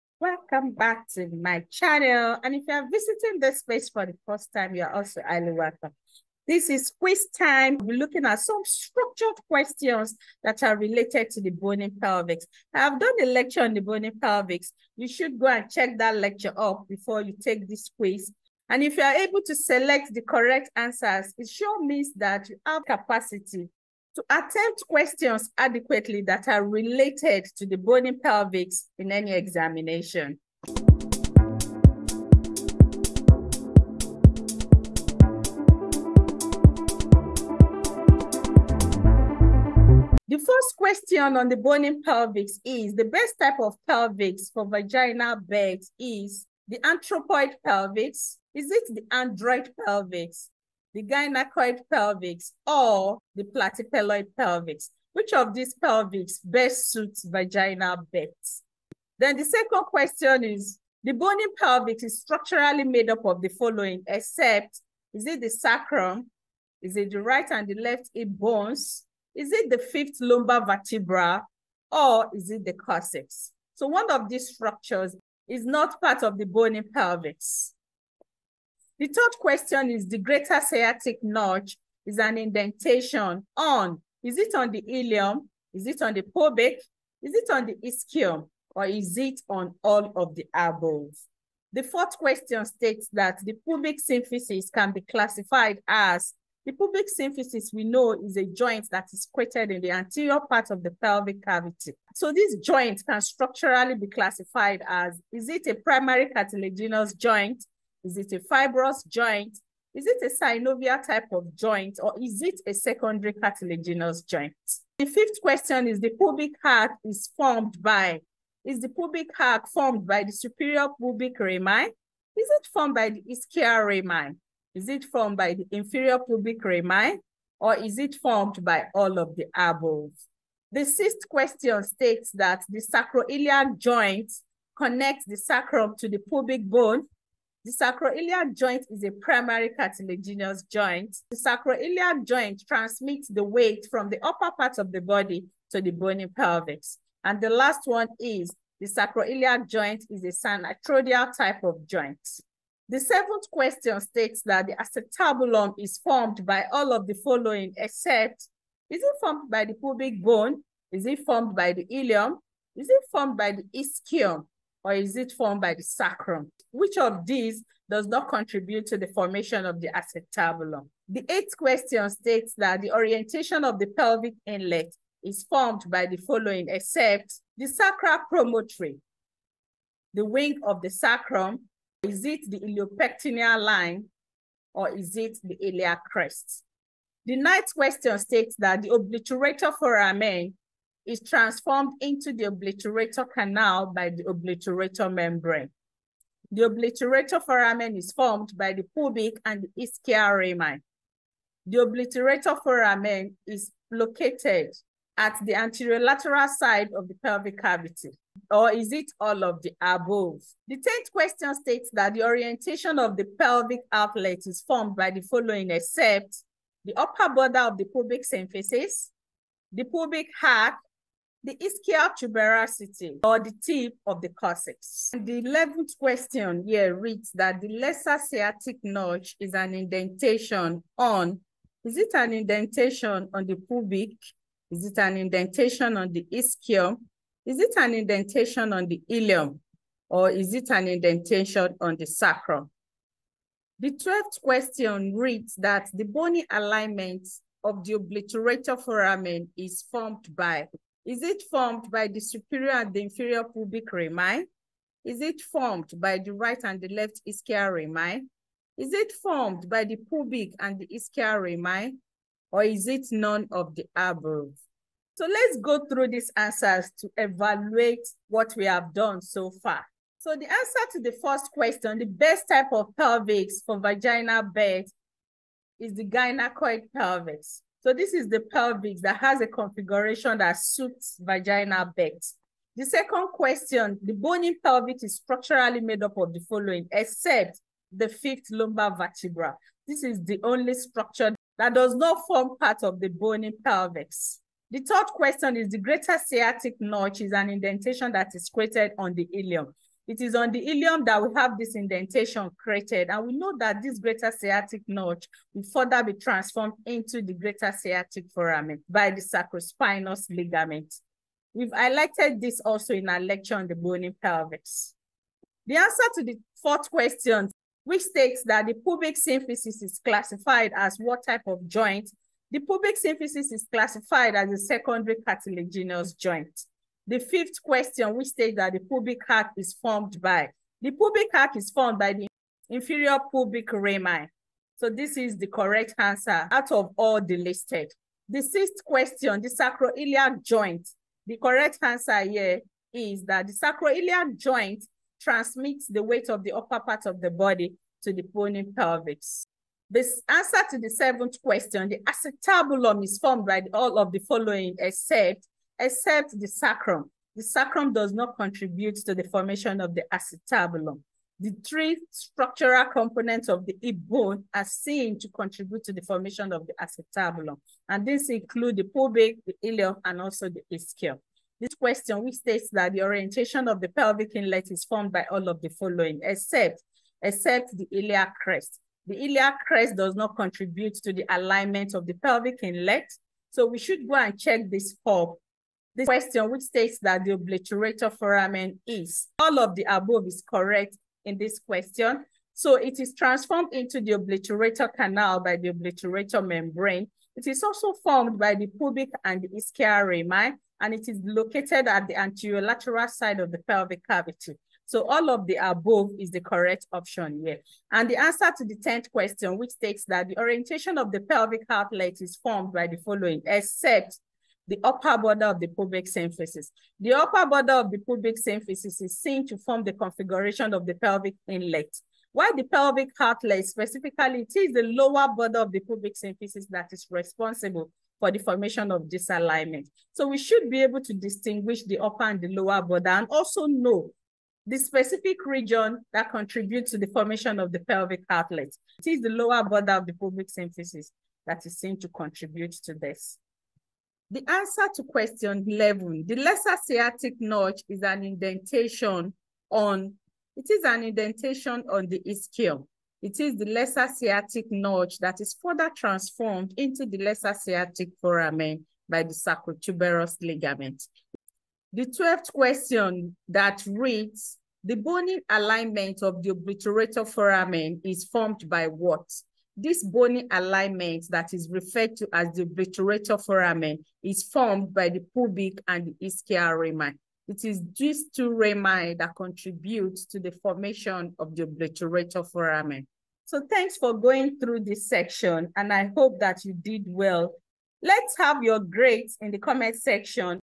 Quiz on the bony pelvis
Check questions on the bony pelvis to evaluate your preparedness for exams